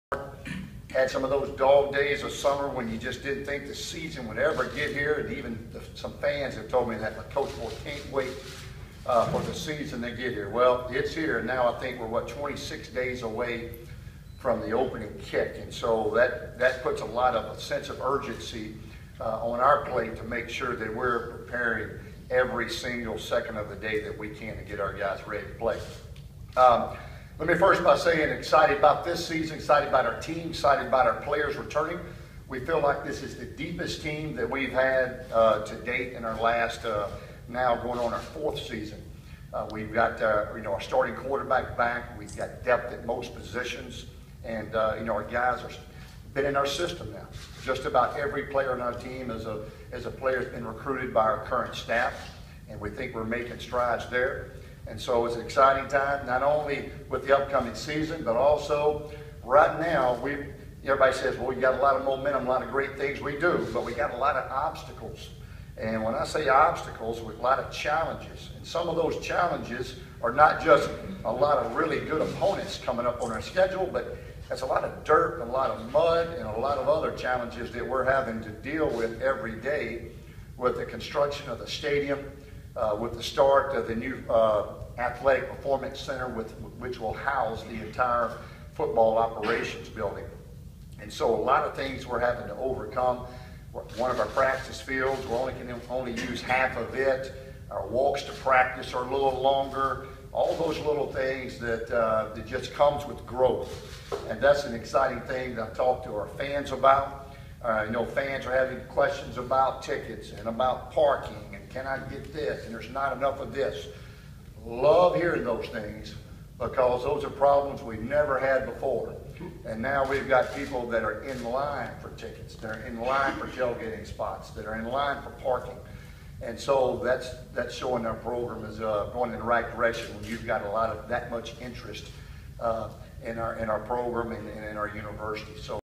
UL-Lafayette coach Mark Hudspeth talks about Cajun fans lining up for tickets
The University of Louisiana-Lafayette football team opened practice Monday and also held media day. Ragin&apos; Cajuns coach Mark Hudspeth, whose squad has been selected to win the Sun Belt Conference, talks about expectations and obstacles heading